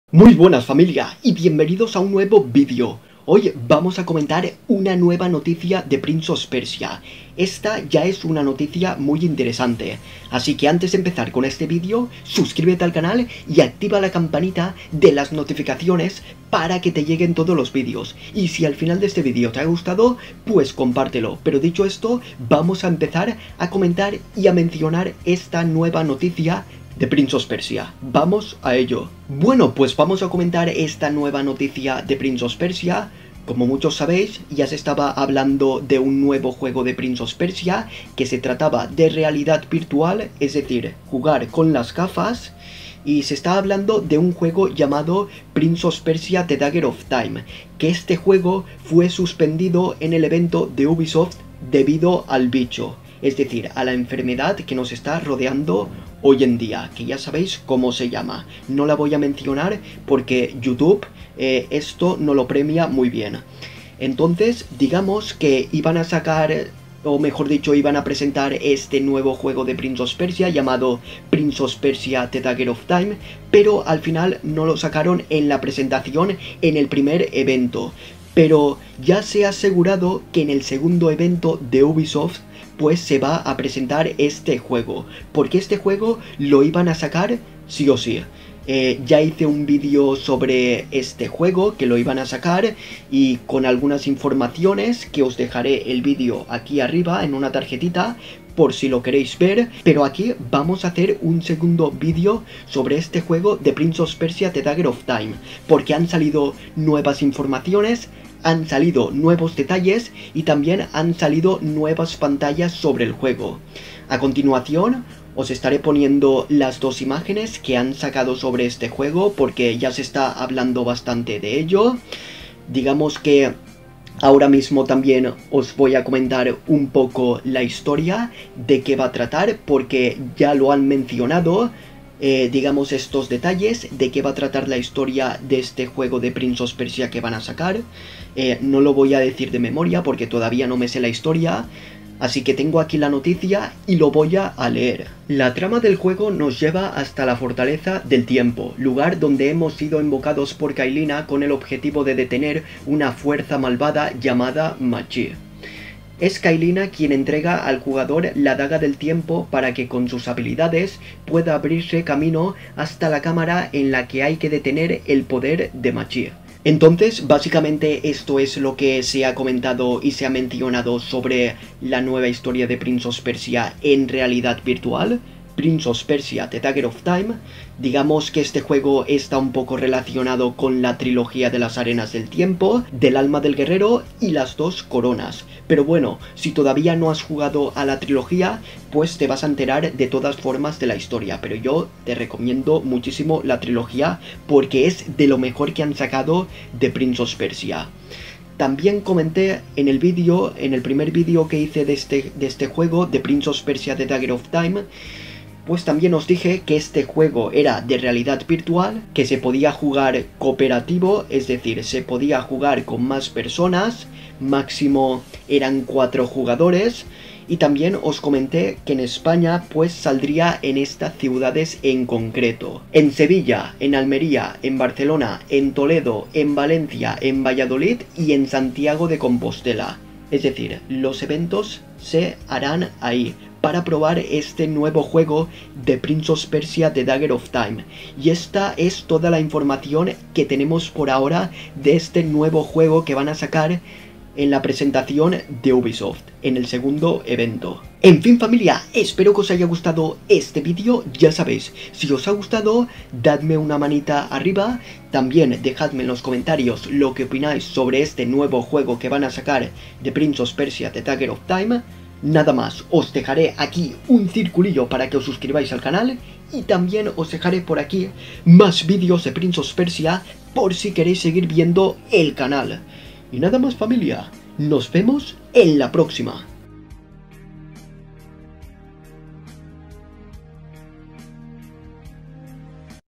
💥 TODO lo que SABEMOS de PRINCE OF PERSIA: THE DAGGER OF TIME (lanzamiento, detalles y más) 🗡
En este vídeo voy a comentar la última noticia de el juego Prince of Persia the Dagger of Time que saldrá en el segundo evento de Ubisoft Forward 2020. Comentaré los nuevos detalles que han salido.